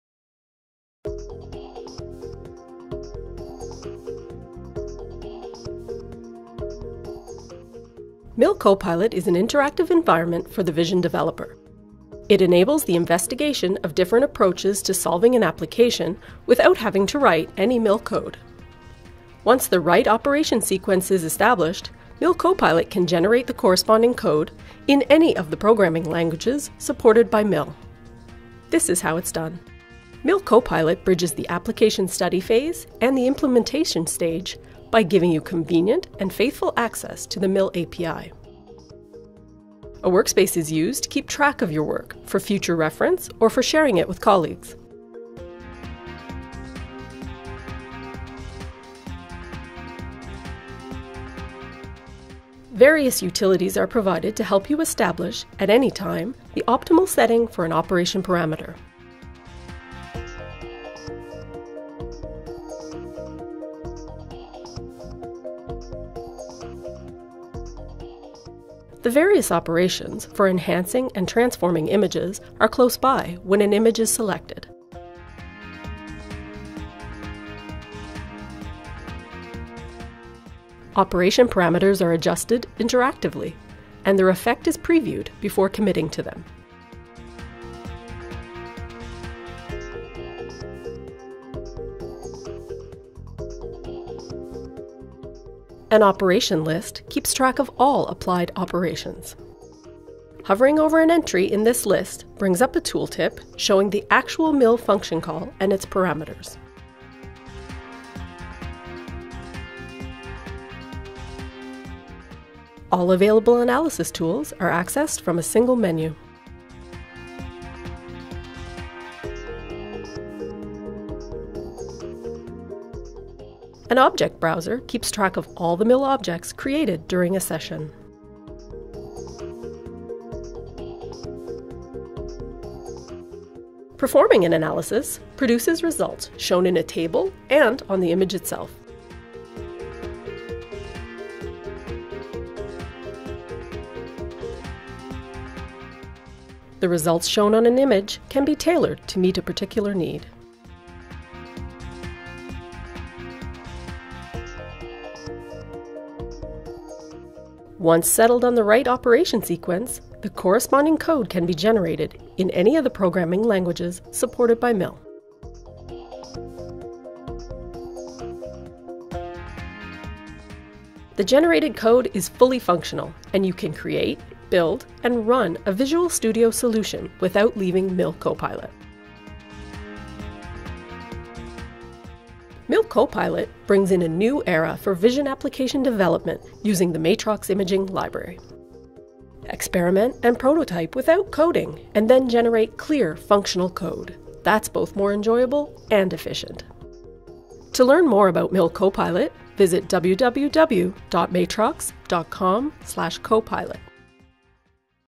Matrox MIL10 新增套件工具集合原型：MIL CoPilot
在windows 64位元的作業系統下,MIL CoPilot提供一個交互式的環境,讓開發人員在程式開發前,可更輕鬆與快速的編譯程式。

G4 Technology